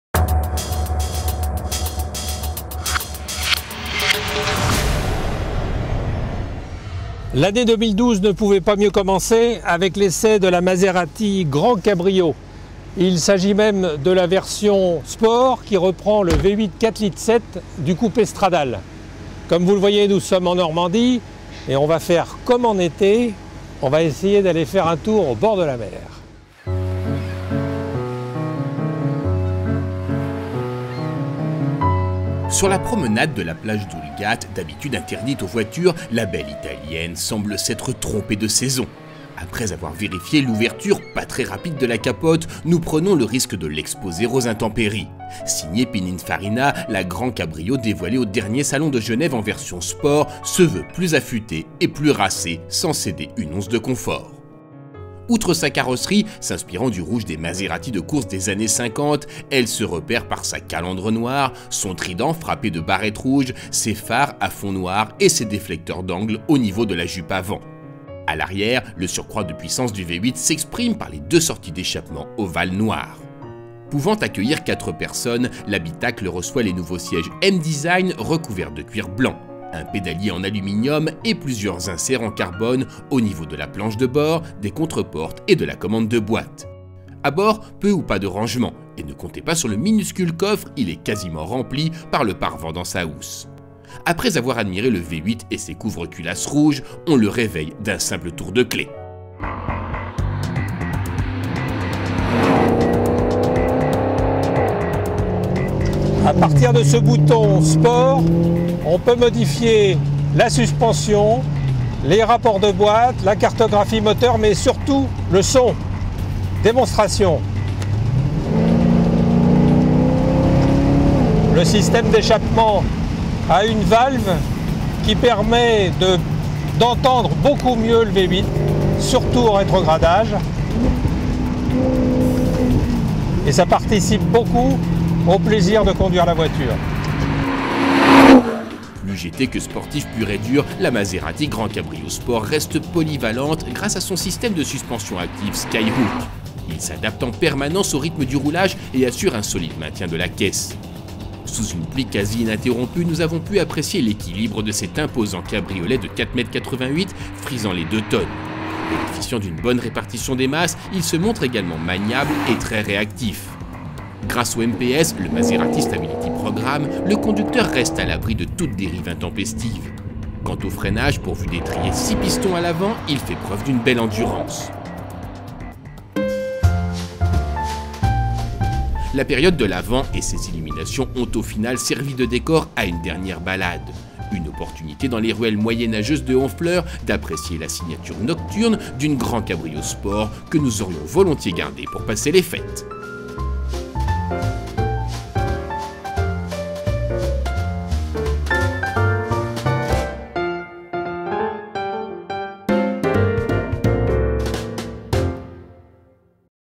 Essai Maserati GranCabrio Sport (2012)
En empruntant au Coupé MC Stradale son V8 porté à 450 chevaux, la Maserati GranCabrio Sport augmente ses performances tout en gardant son agrément de GT confortable.